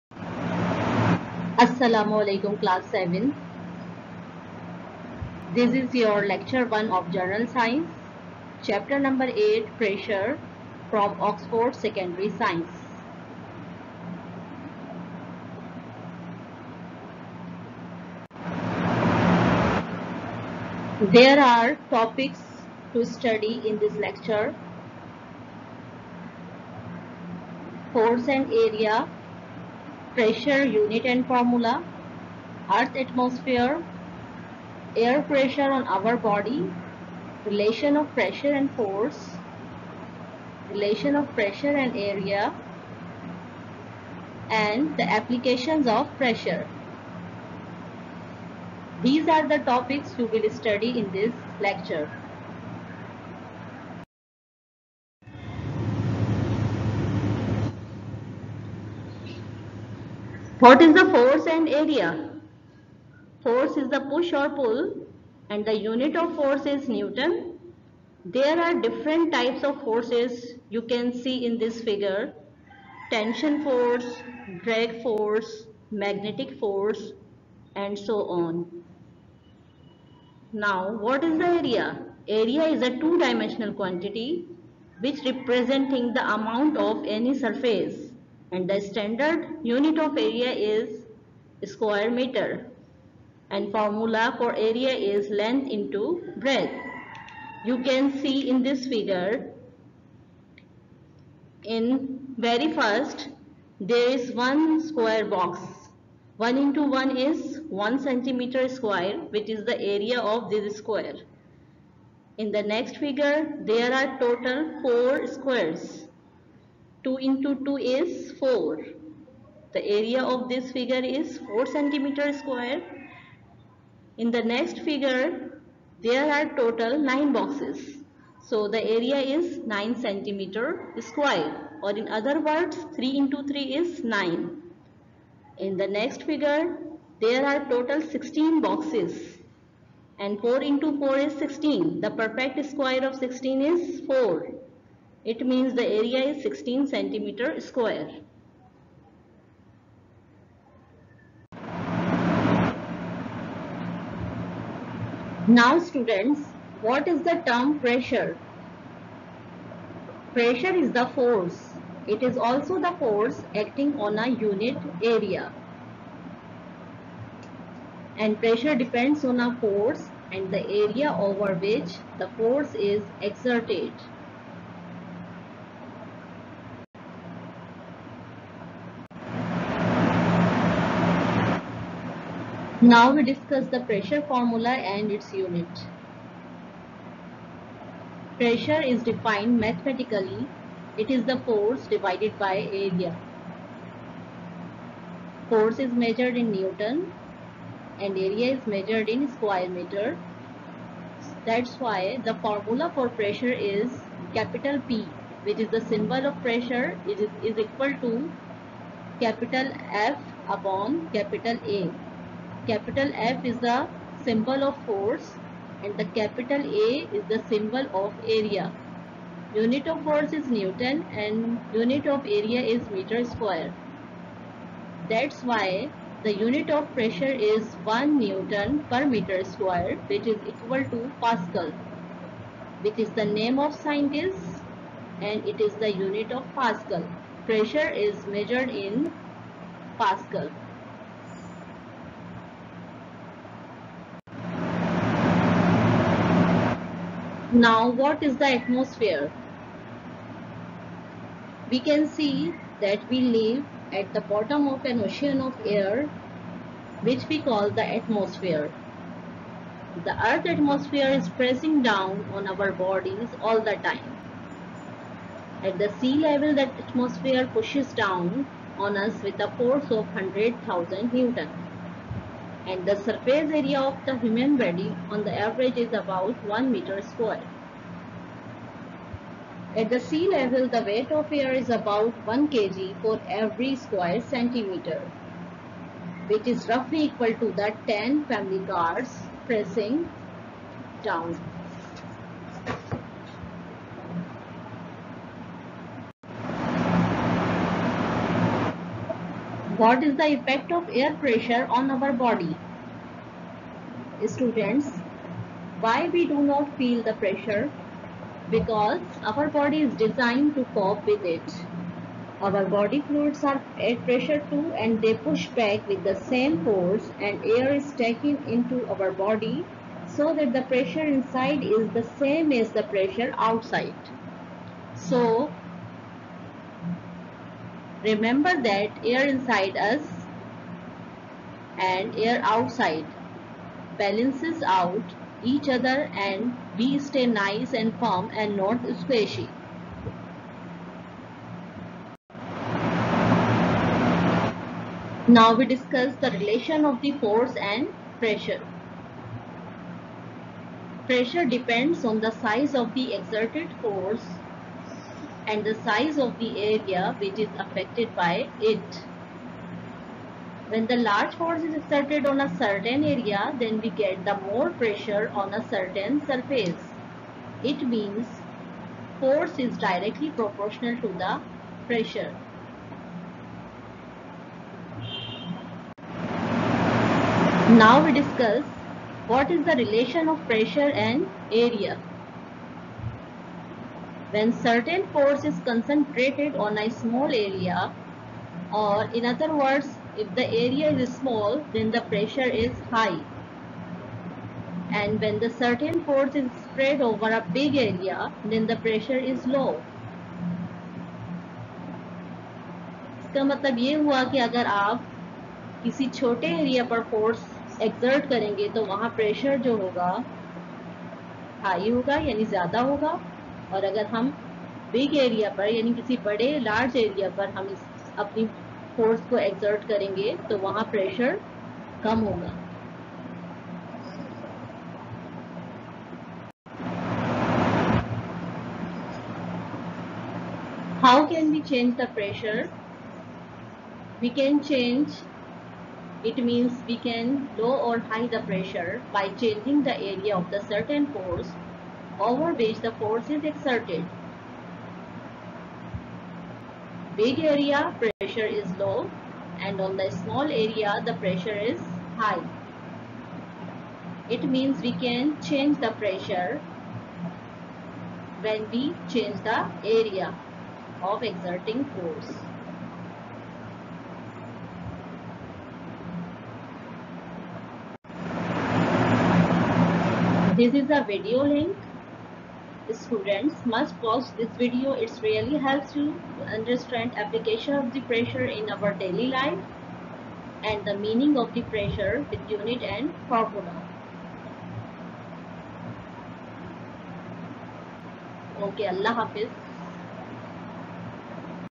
lecture 1 Physics, chapter 8, Pressure
this video contents are force, relation area, pressure with unit and formula ,application of pressure, relation of force and pressure as well relation of area and pressure with examples and figure.
a video link has been attached with this video.